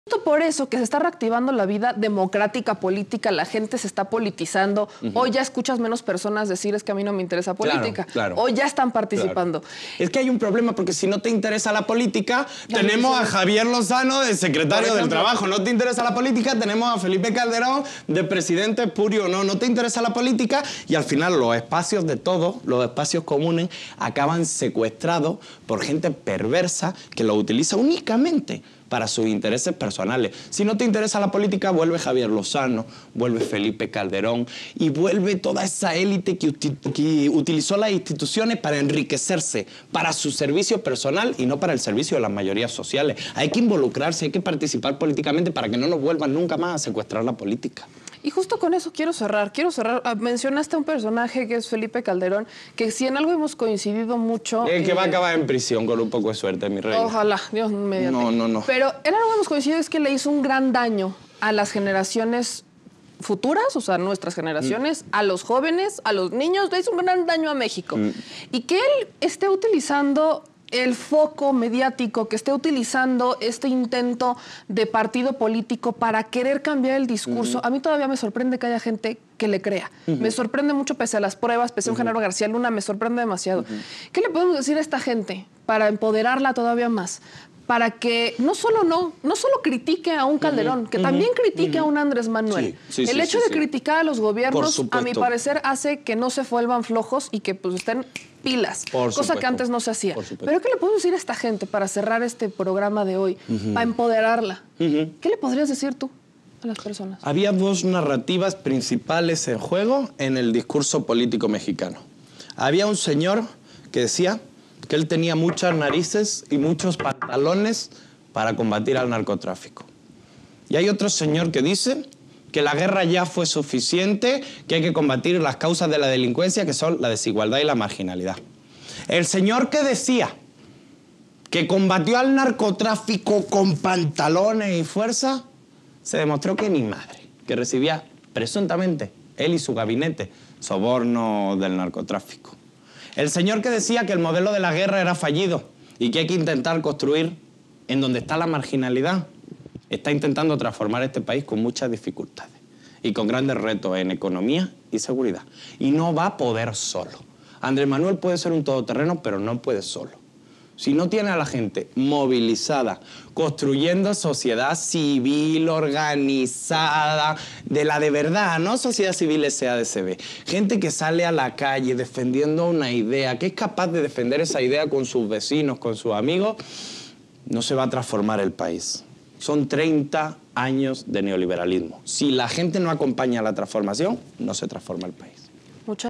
ABRAHAM MENDIETA LO DICE CLARO: ¡PORQUE NO PODEMOS CREER EN CALDERÓN!
Hay dos tipos de presidentes, con dos tipos de discursos ¿en cuál confías más?